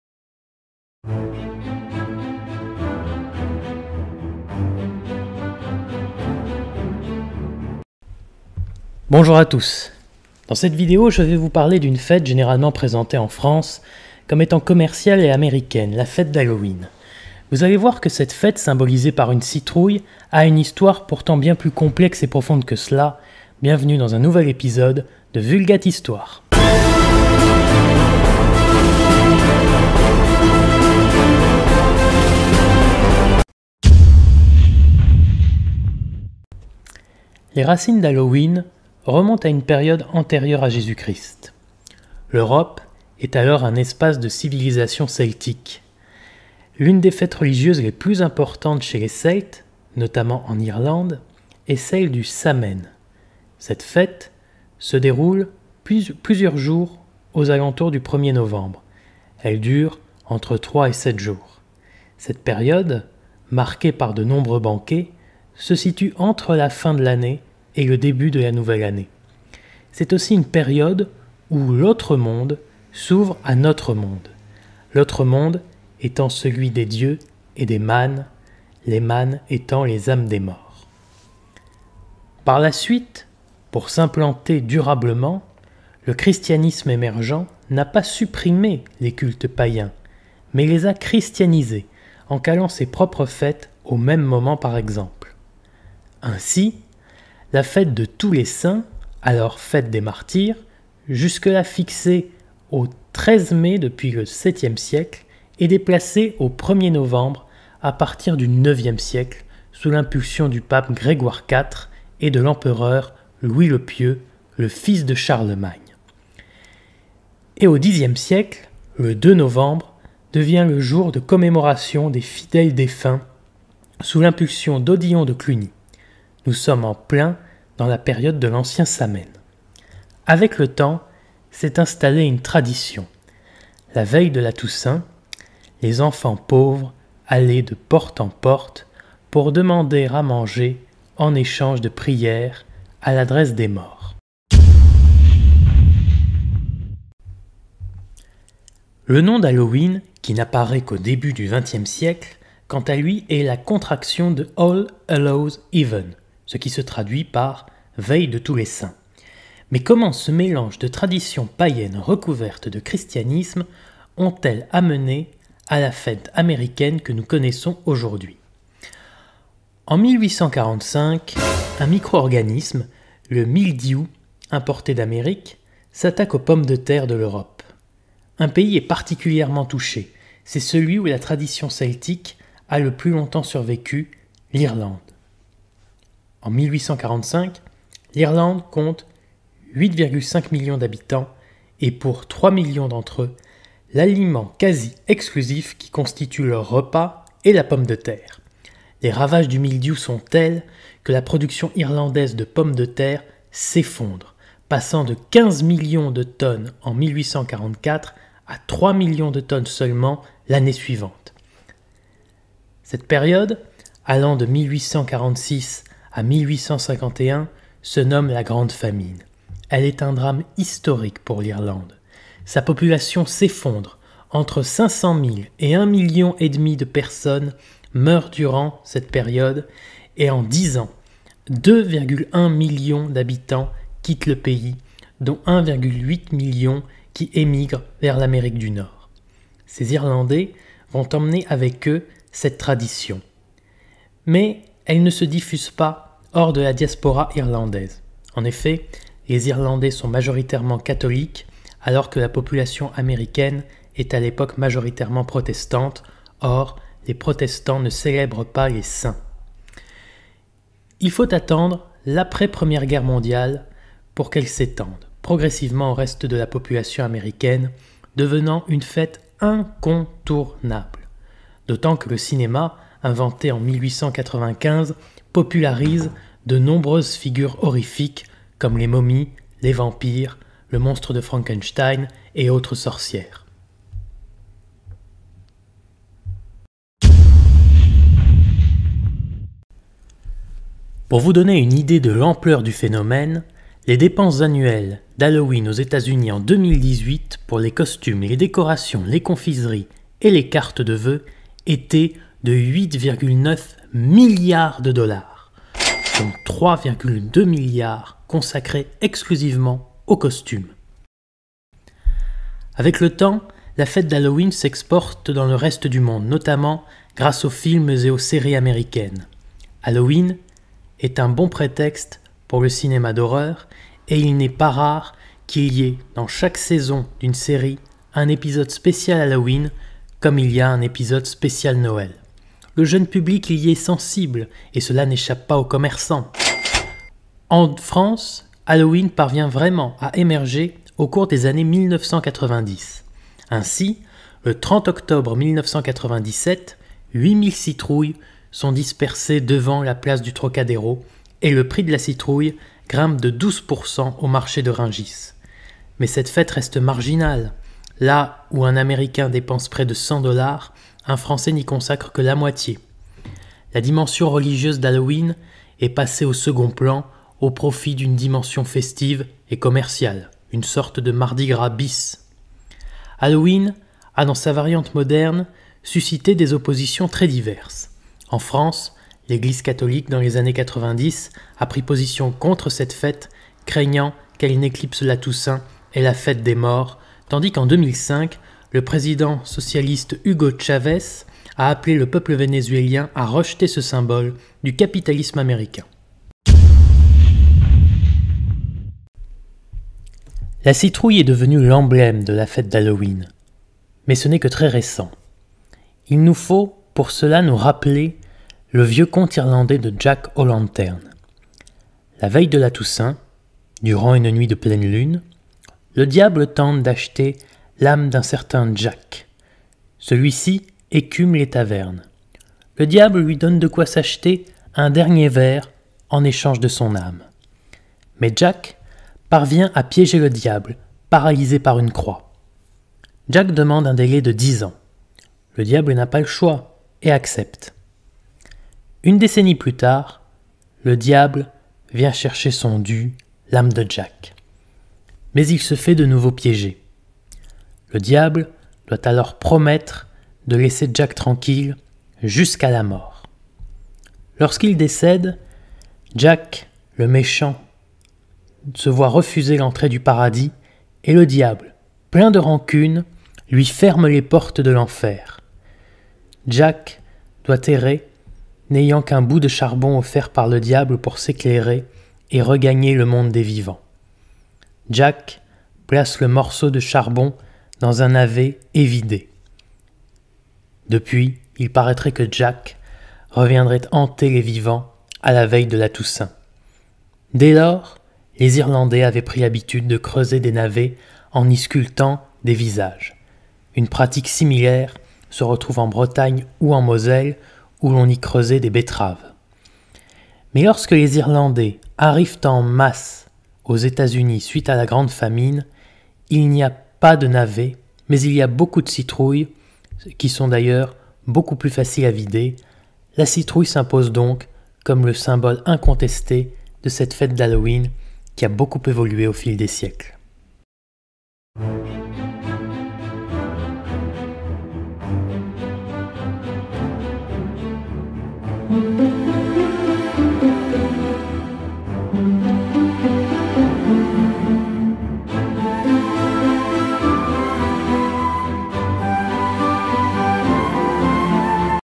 D'où vient Halloween? Citrouilles et Pommes de terre
Halloween a la réputation d’être une fête américaine purement commerciale. Ça n’est pas faux, mais saviez vous qu’à l’origine Halloween provient d’une des plus importantes fêtes du monde celte, le Samain ? Le Samain comme de nombreuses fêtes païennes a ensuite été christianisé. Puis les traditions Irlandais ont été exporté aux Etats-Unis durant le XIXe siècle lorsque la production de pommes de terre du pays d’effondre, forçant près de 2 millions d’Irlandais à quitter le pays.

DECOUPAGE de l’épisode
00:00  Introduction
00:35  Partie 1, Aux origines
02:25  Partie 2, L’américanisation d’une tradition
04:47 : Partie 3, Halloween en Amérique et dans le monde
06:36 : BONUS : Pourquoi une citrouille ?

Sources :
Bibliographie :
BENSIMON Fabrice COLANTONIO Laurent : La grande famine en Irlande, 2014, Armand Colin

MUSIQUE
Musique proposée par La Musique Libre